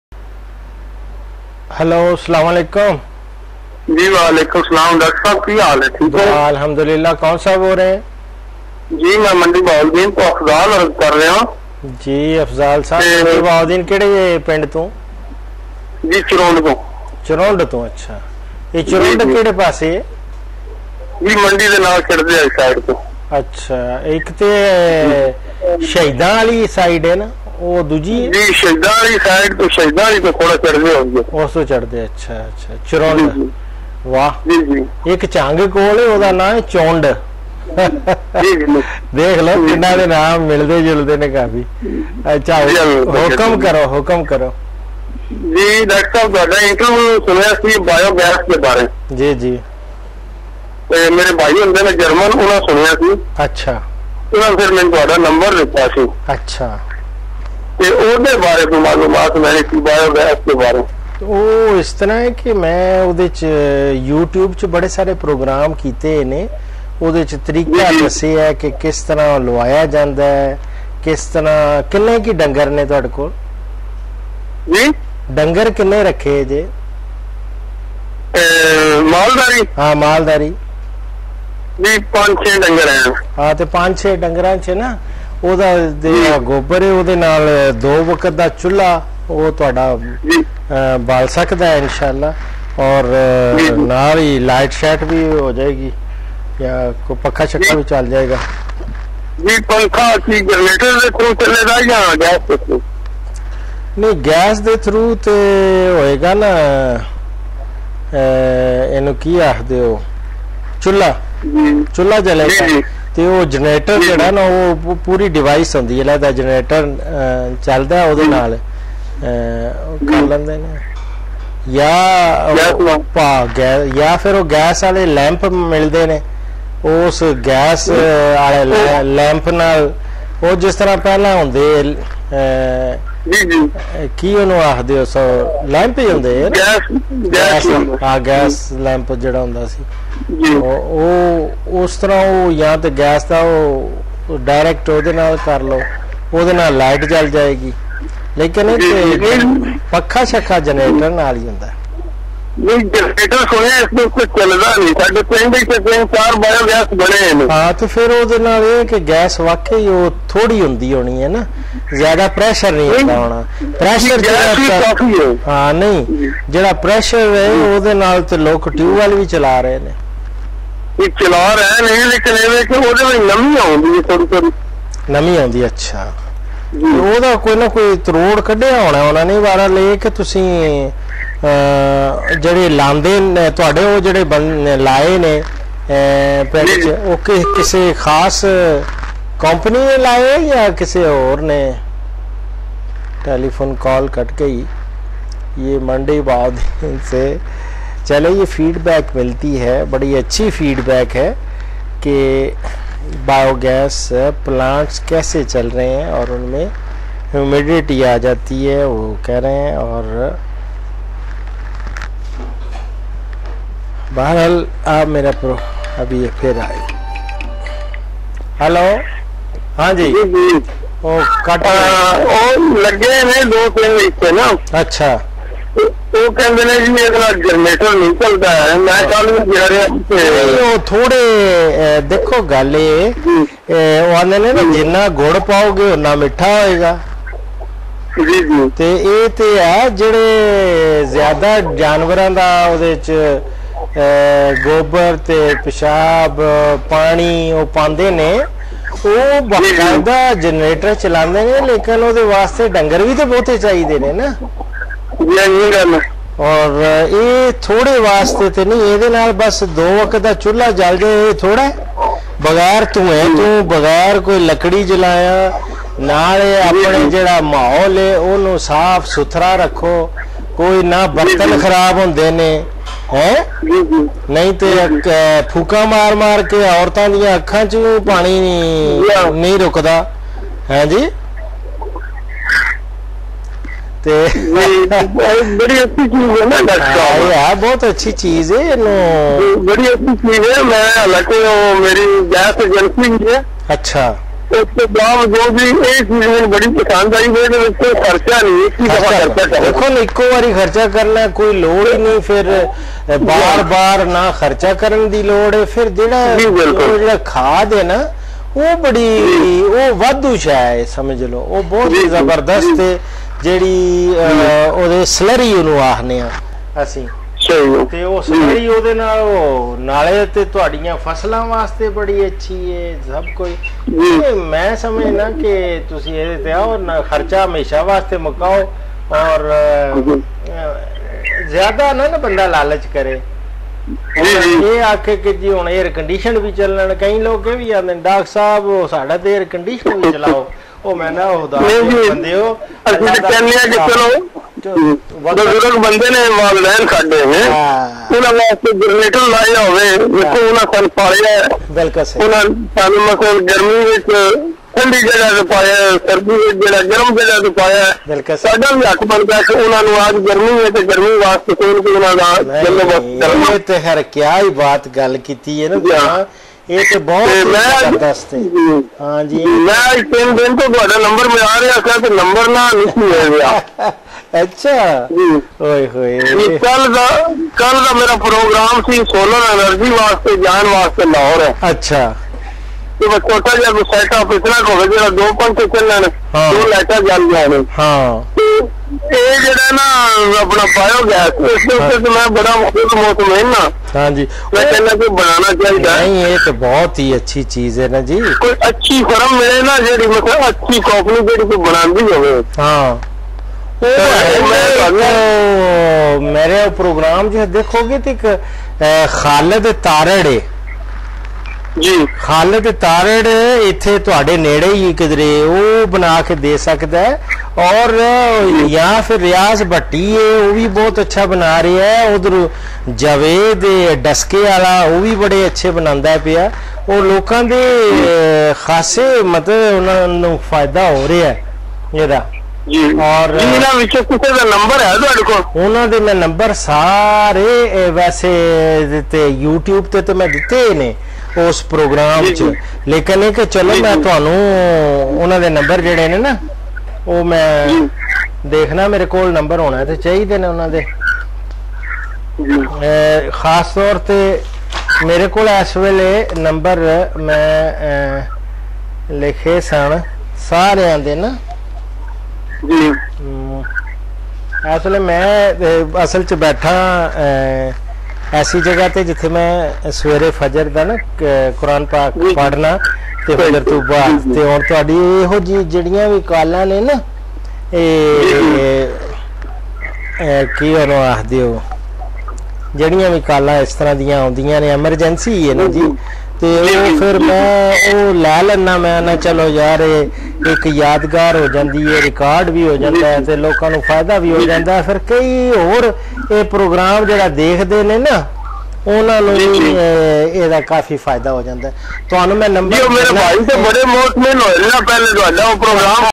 BIOGAS MANDI BAHAUDDIN PUNJABI DR.ASHRAF SAHIBZADA .wmv
DR.ASHRAF SAHIBZADA (a native of BHADDAR GUJRAT) is a world renowned Pakistani Agricultural Scientist. He extends free advisory service on all aspects of agriculture & livestock to famers of Pakistan as a noble deed.